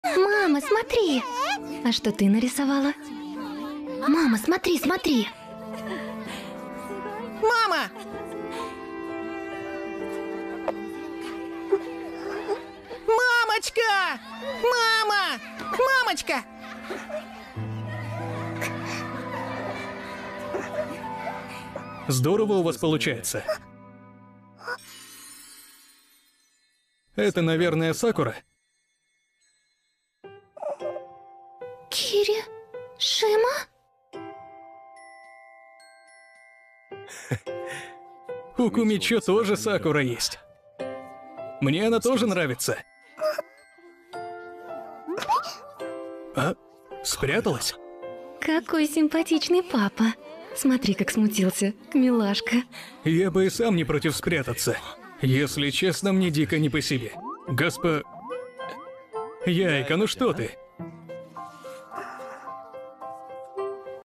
Никто не приходил посмотреть на её рисунки,но в последний момент...
Аниме